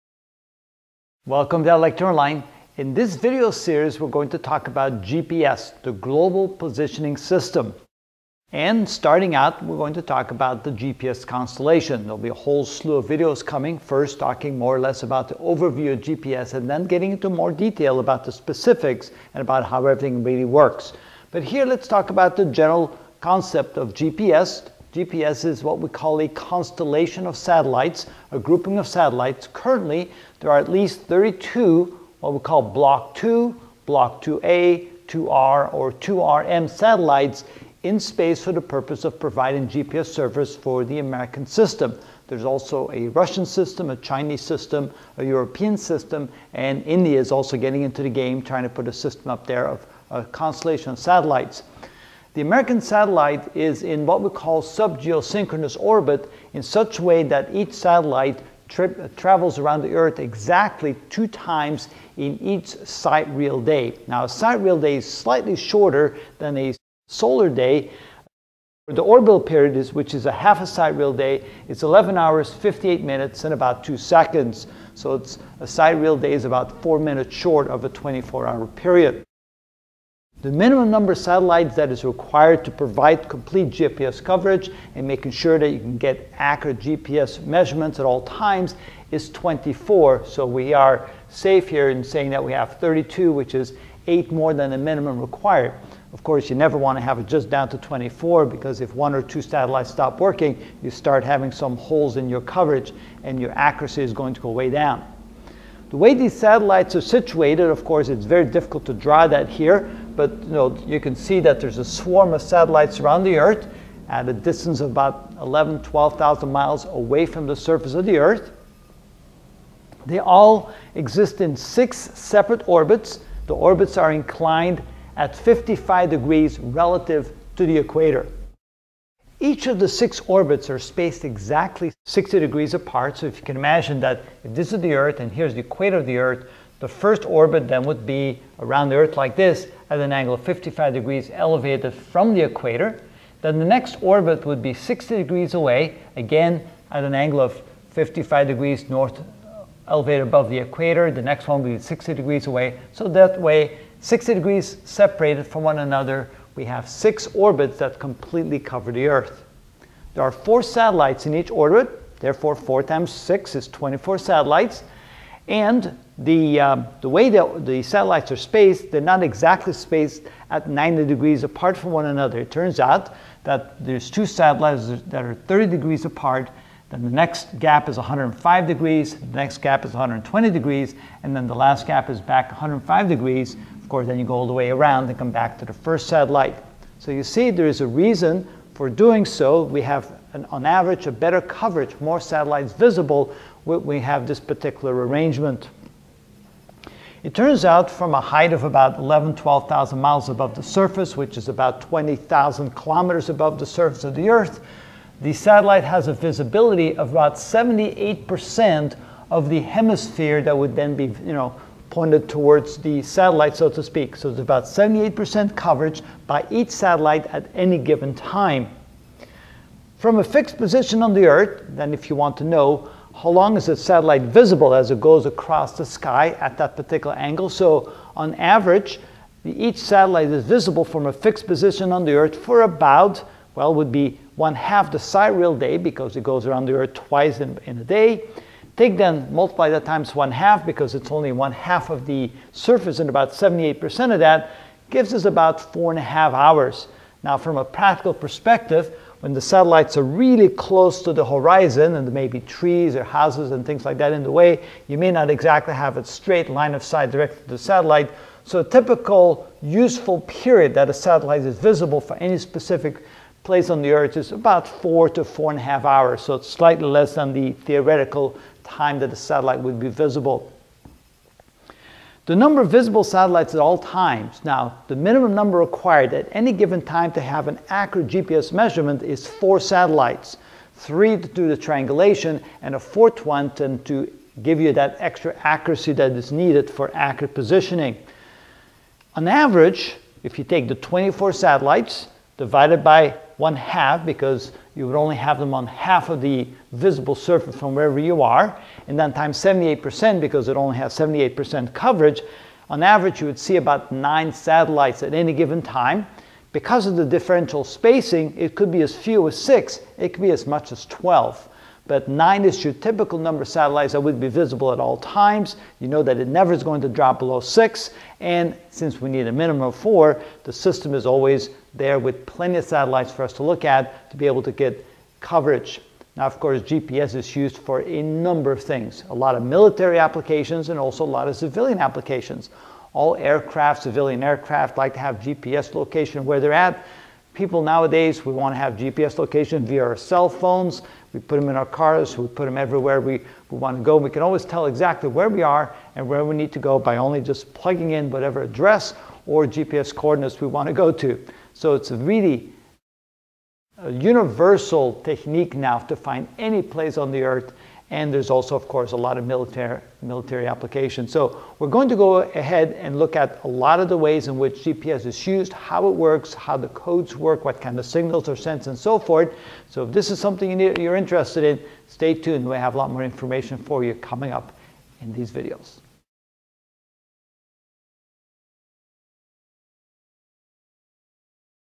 Special Topics - GPS (1 of 100) The GPS Constellation
In this video I will overview the content of the GPS (Global Positioning System) and explain the GPS constellation.